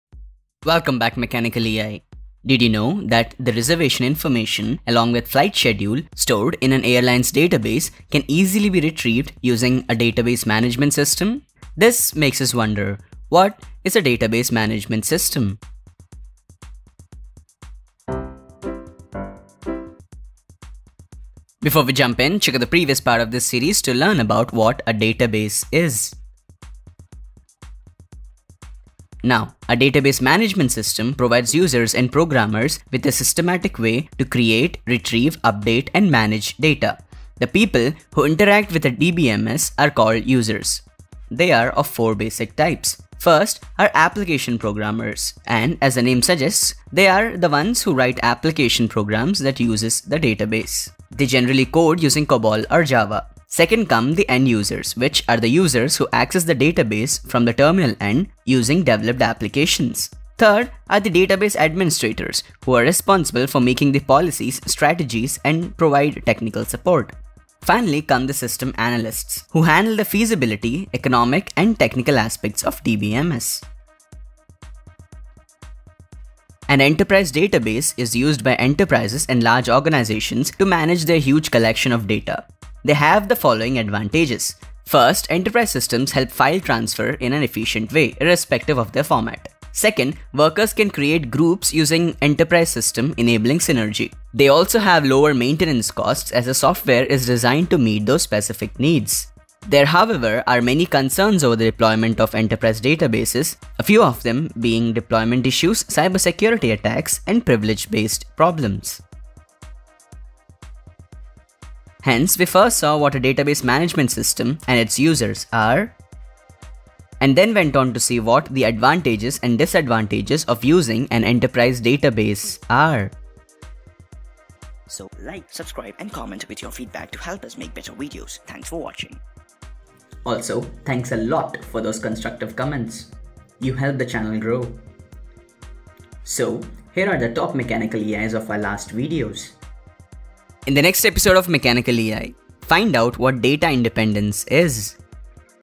What is a Database Management System? Users of DBMS | Pros and Cons of Enterprise Database
Welcome back MechanicaLEi, did you know that the reservation information along with flight schedule stored in an airline's database can easily be retrieved using a Database management system? This makes us wonder, What is a Database Management System?
Before we jump in check out the previous part of this series to learn about what a database is? Now, A database management system provides users and programmers with a systematic way to create, retrieve, update and manage data. The people who interact with a DBMS are called users. They are of four basic types: First are application programmers and as the name suggests they are the ones who write application programs that uses the database. They generally code using COBOL or JAVA. Second, come the End users, which are the users who access the database from the terminal end using developed applications. Third are the Database Administrators who are responsible for making the policies, strategies and provides technical support. Finally comes, the system analysts who handle the Feasibility, economic and technical aspects of DBMS. An enterprise database is used by enterprises and large organizations to manage their huge collection of data. They have the following advantages: First, Enterprise systems help file transfer in an efficient way, irrespective of their format. Second, Workers can create groups using the enterprise system enabling synergy. They also have Lower Maintenance Costs as the software is designed to meet those specific needs. There however are many concerns over the deployment of enterprise databases a few of them are Deployment, Cybersecurity attacks, and Privilege-based issues. Hence, we first saw what a database management system and it's users are and then went on to see what the advantages and disadvantages of using an enterprise database are?
In the next episode of MechanicaLEi find out what Data Independence is?

All the images and audio are the copyrights of their respective organizations/individuals. The issue will be resolved in a few working days.